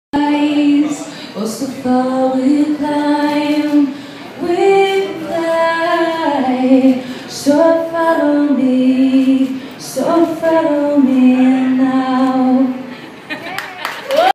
Emma Hewitt Sings during MLD Night Out Press Conference
She will perform in 12 cities across Indonesia as a part of Djarum Super Mild Night Out tour.